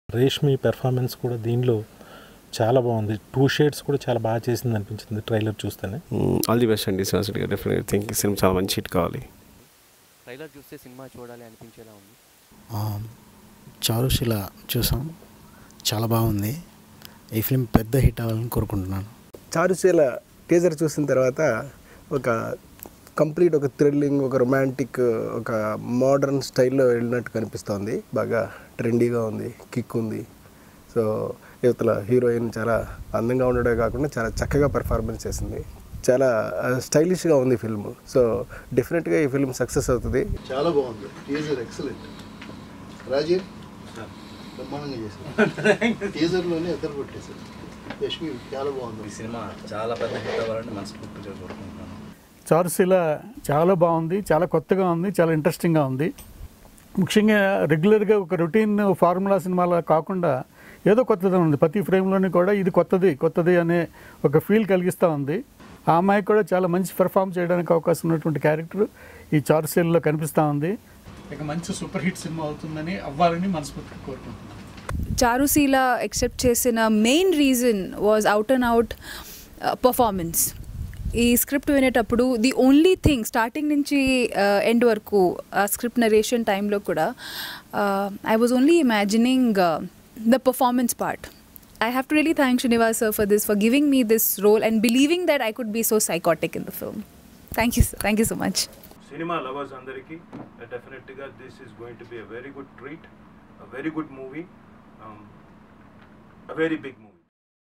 Tollywood Top Directors Best Wishes to Charu Seela Movie Team - Rashmi || Rajiv || Srinivasa Reddy
Watch Tollywood Top Directors Best Wishes to Charu Seela Movie Team
Starring : Rashmi Gautham, Brahmanandam, Rajiv Kanakala, Jaswanth
Producers : V Sagar
Director : V Srinivasa Reddy